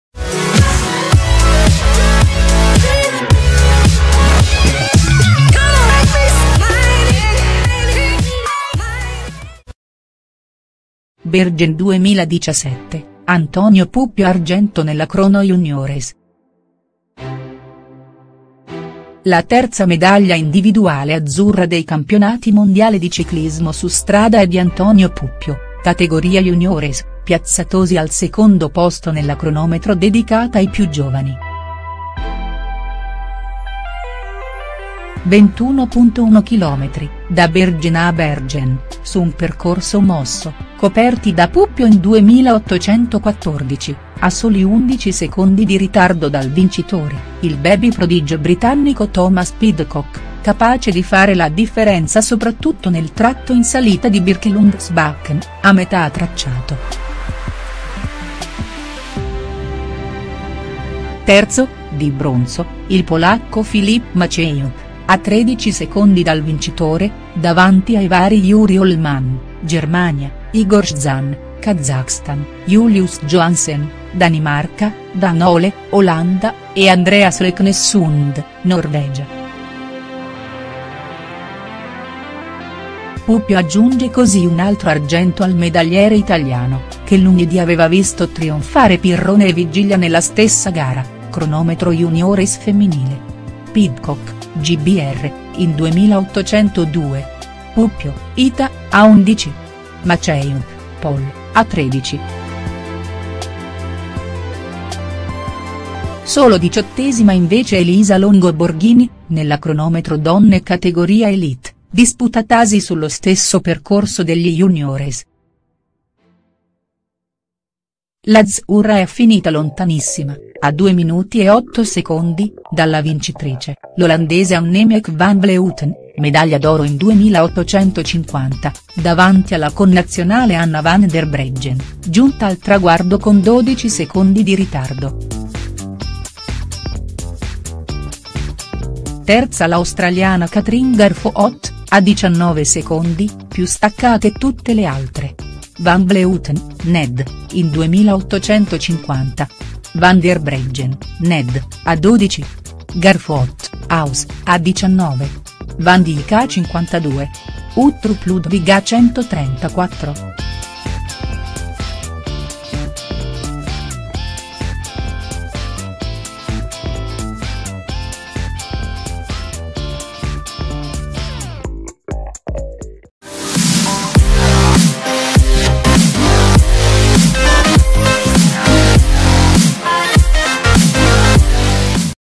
Bergen 2017, Antonio Puppio argento nella crono juniores.HD
L'azzurro chiude secondo nella cronometro juniores vinta dal britannico Pidcock. Lontana dal podio Elisa Longo Borghini nella prova contro il tempo élite donne.